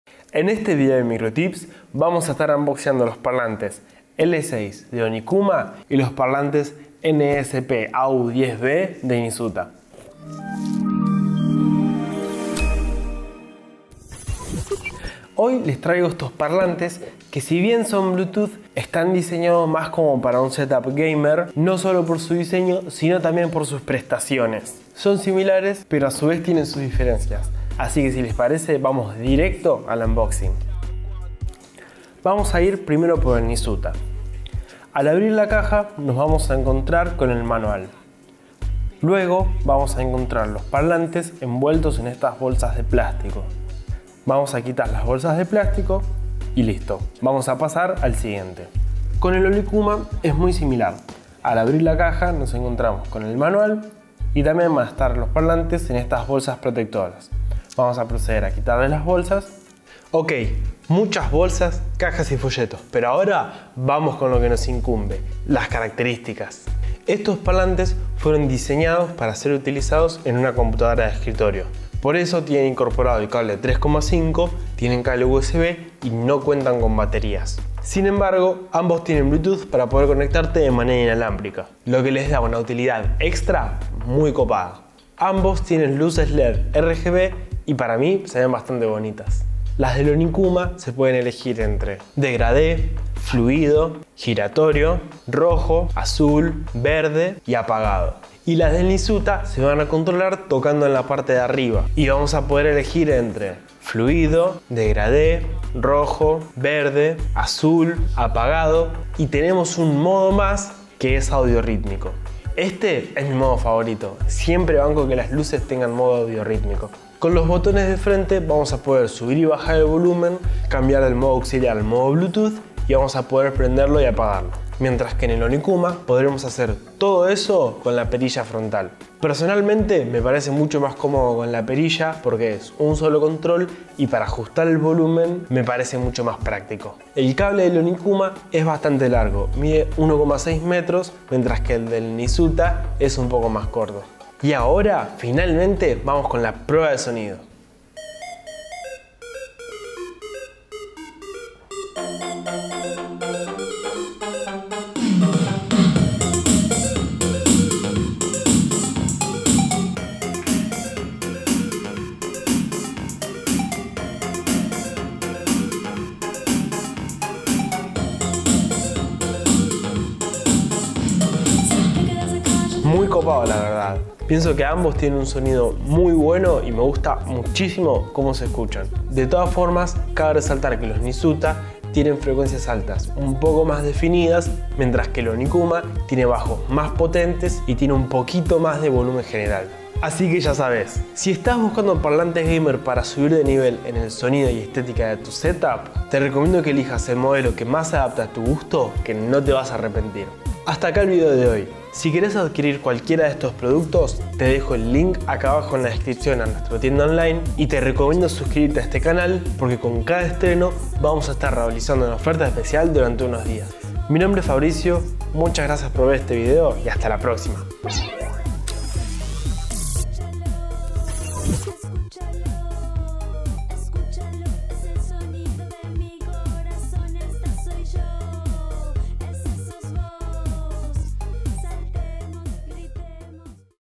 Parlantes Bluetooth Onikuma Vs. Nisuta ¿Cuál es mejor?
Mirá todas las características y diferencias de los parlantes Onikuma L6 y Nisuta NSPAU10B

Gran parte de la música de nuestros vídeos podés escucharla en mi canal personal 🎧